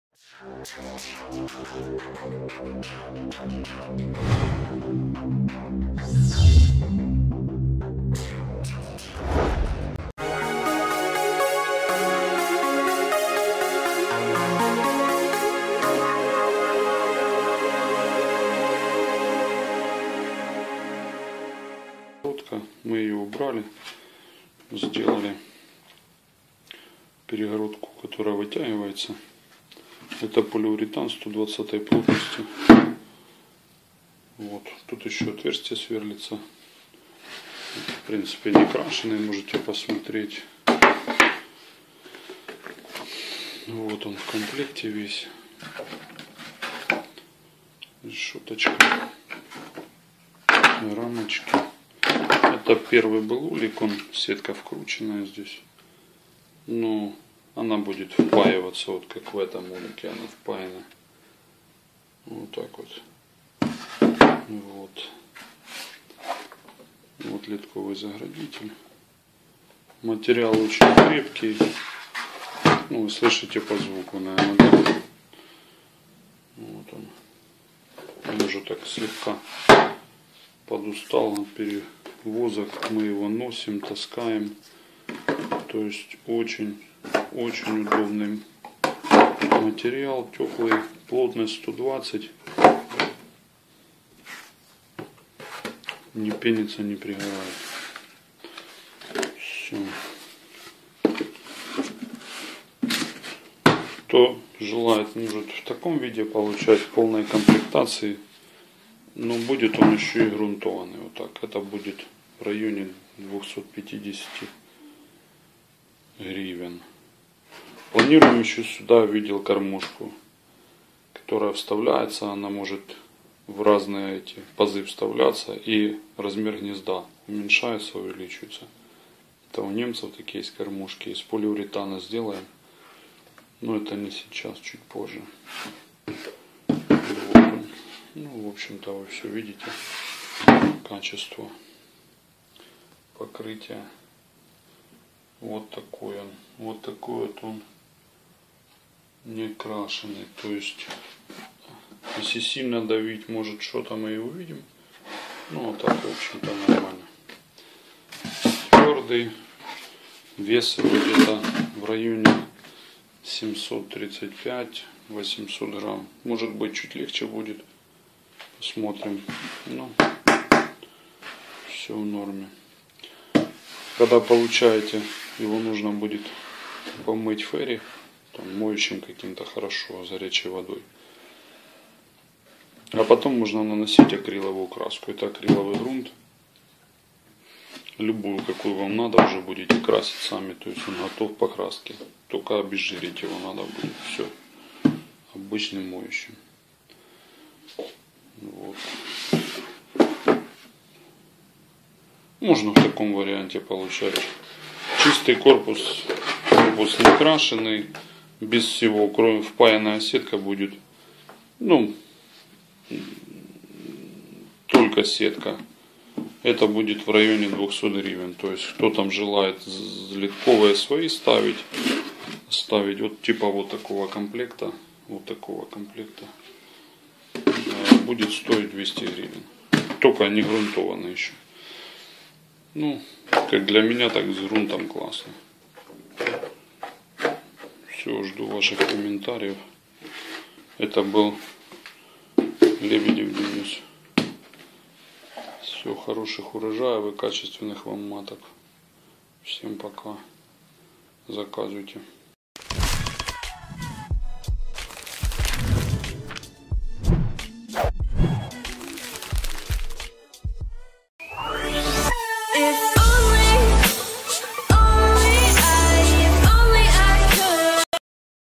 Нуклеус из ППУ & Изготовление нуклеуса под заказ
Денис Лебедев изготавливает нуклеус на одно маткоместо . Материал- полиуретан, плотность 120-100 едениц цена комплекта с впаяным грунтом, рамками 250-270 гривень. Тел.

🐝Друзья спасибо Вам большое за внимание!🐝
🐝Friends thank you very much for your attention!🐝